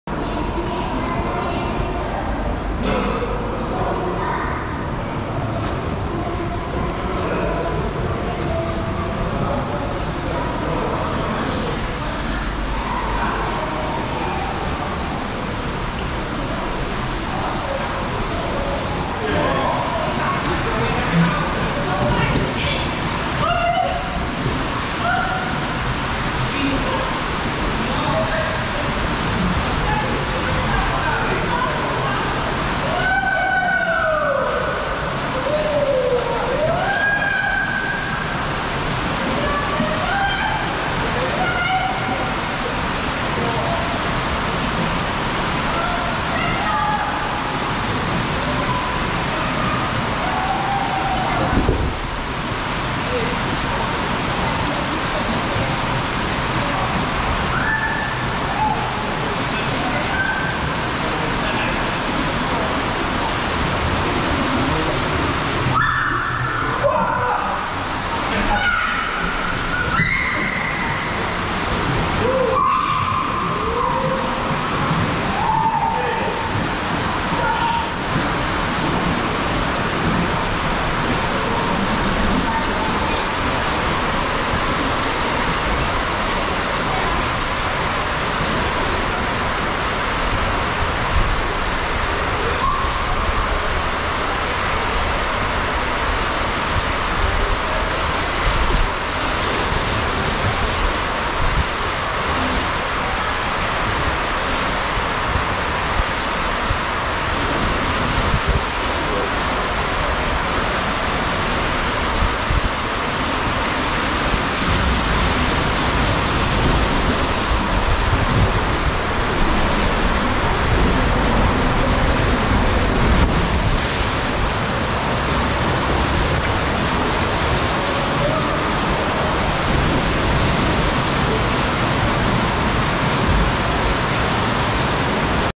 Chochołowskie Termy - The Blue Infinity Water Slide
The Blue Infinity Waterslide Ride at Chochołowskie Termy, a indoor water park located at Polands largest Thermal Baths the ‘ Chochołowska Baths ' in Chocholów, Witów, Poland